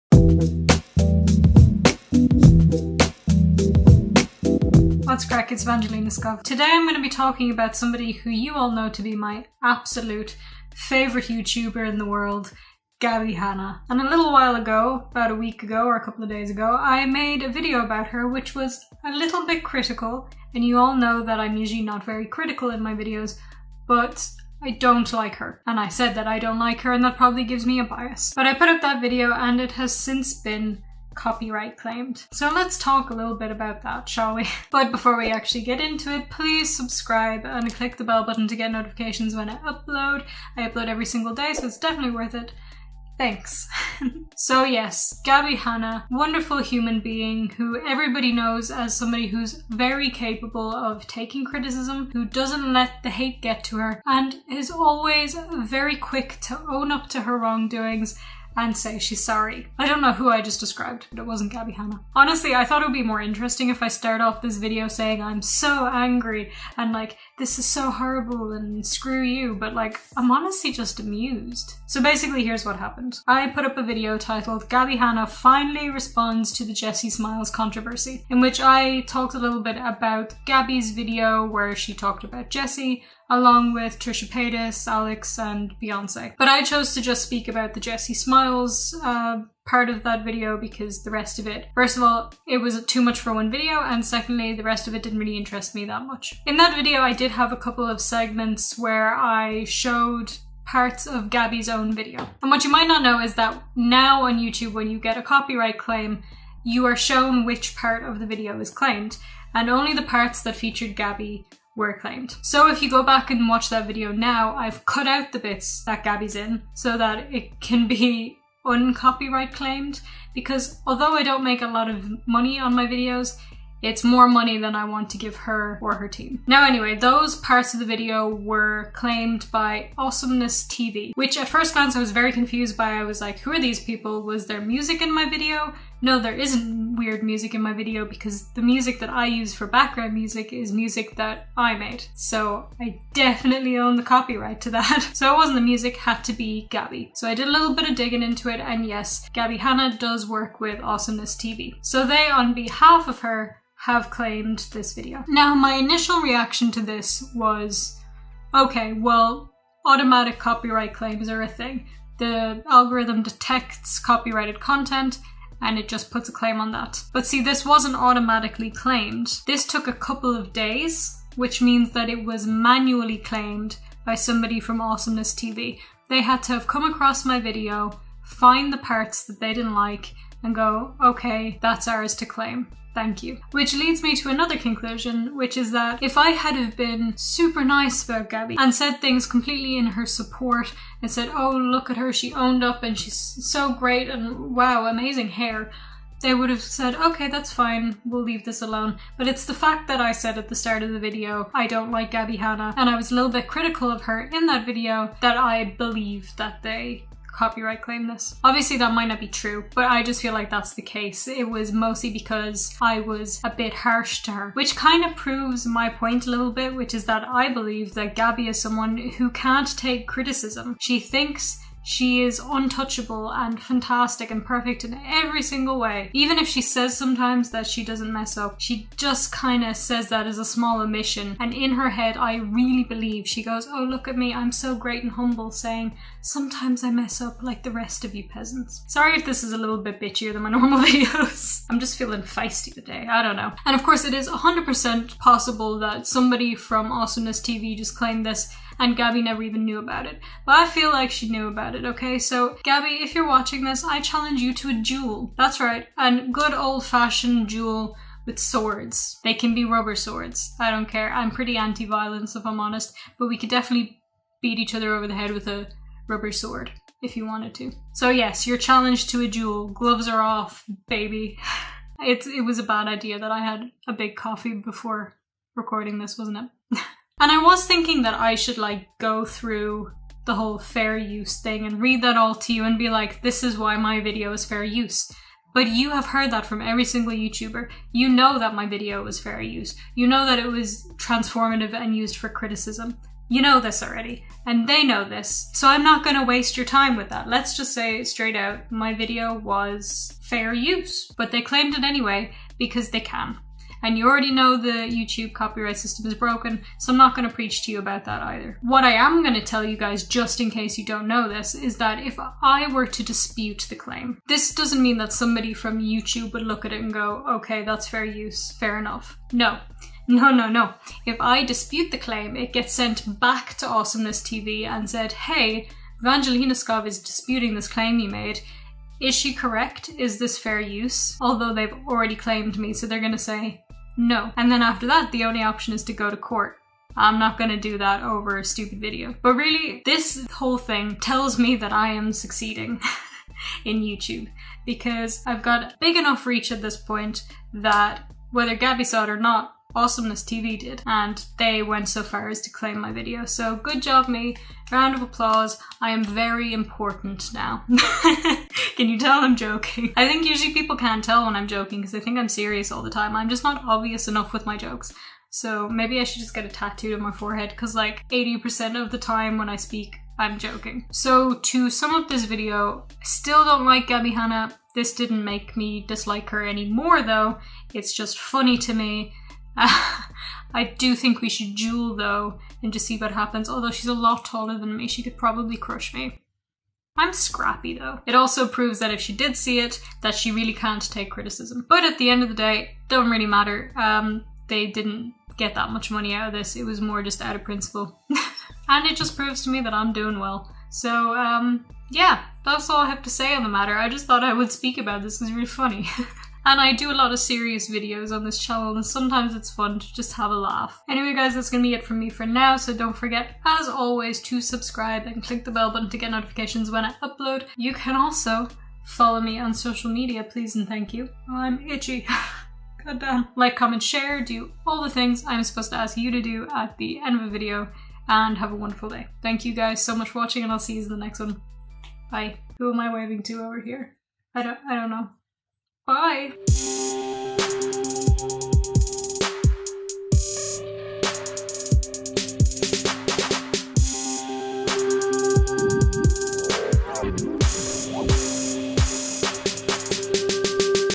Gabbie Hanna Copyright Claimed MY Video
Gabbie Hanna (with AwesomenessTV), my FAVOURITE YouTuber, copyright claimed my video and now we're going to duel. So, @TheGabbieShow when's the fight?